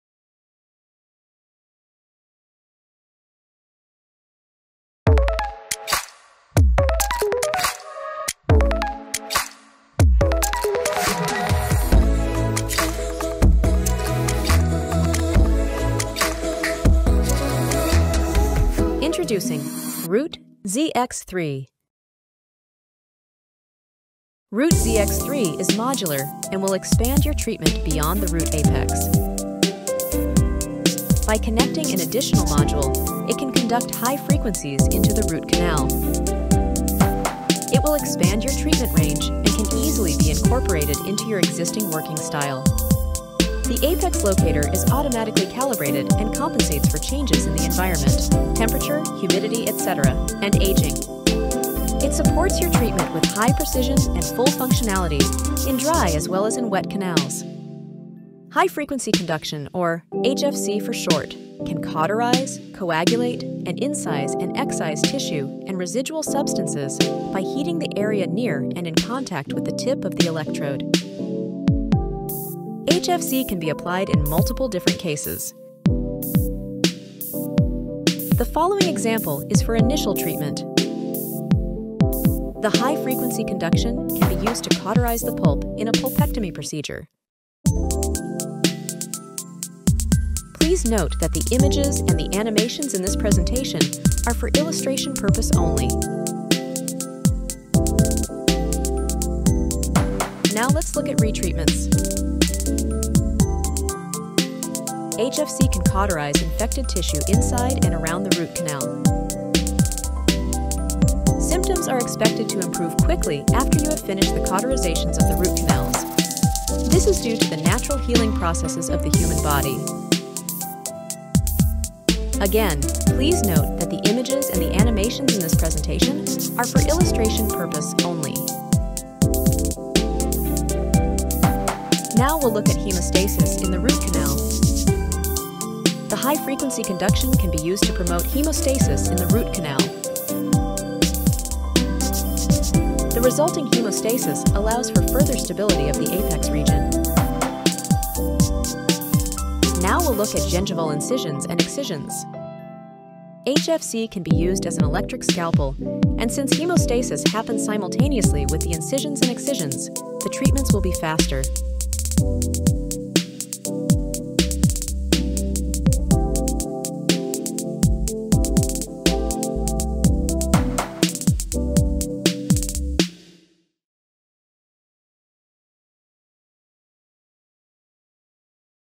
Root ZX3: A New Approach to Canal Treatment
Root ZX3 is a new approach to canal treatment and goes beyond conventional canal shaping and cleaning.

All-new high-frequency conduction therapy can cauterize pulp and promote rapid and effective healing after treatment.

From improved efficacy, to faster treatment times, the Root ZX3 will become an indispensable part of your treatment plan.

Learn what the high-frequency conduction module can do for you and see a whole new approach to root canal therapy.

00:00 Introduction
00:40 Apex Location
00:56 High-Frequency Conduction (HFC)
01:15 Initial Treatment
01:37 Retreatment
02:09 Hemostasis in the root canal
02:27 Gingival Incision and Excision

Discover a new world of root canal treatment.